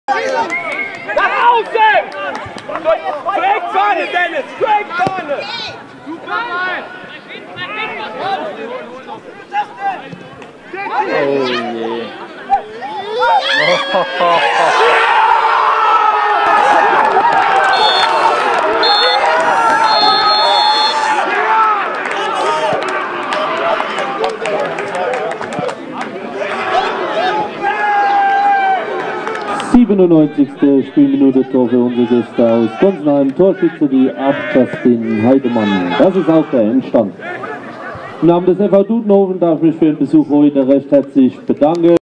Stürmer zeigt dem Torwart seine kalte Schnauze
Meisterschaftsspiel in der Verbandsliga Südwest FV Dudenhofen gegen SV Gonsenheim am Sonntag, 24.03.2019.